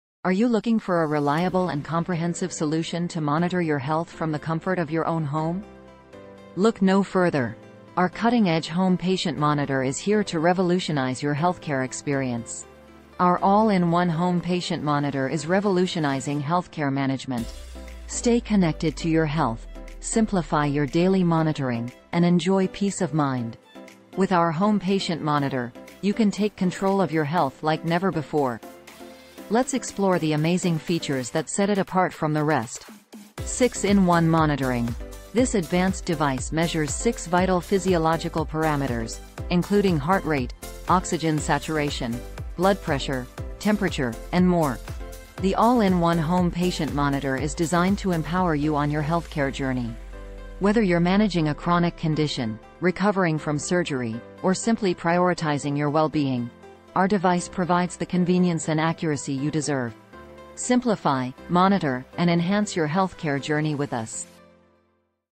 Revolutionize Your Healthcare with Our All-in-One Home Patient Monitor
Discover the revolutionary All-in-One HomePatientMonitor, your comprehensive solution for monitoring your health from the comfort of your own home. Take control of your health like never before with our cutting-edge device.
Shanghai Berry Electronic Tech Co., Ltd. quality manufacturer from China.